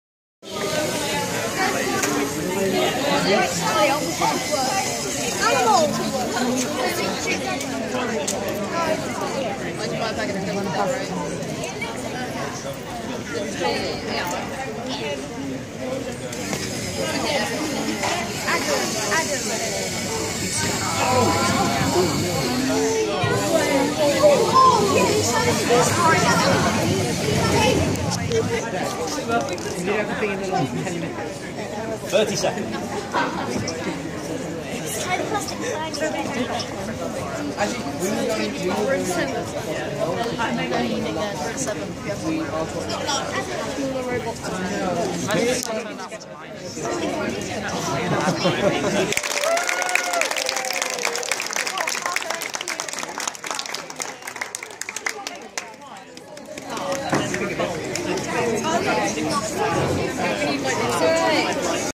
Robot wars at the First Lego League 13/01/16
The children from schools in the South East battle it out to see who will be the winners of the First Lego League.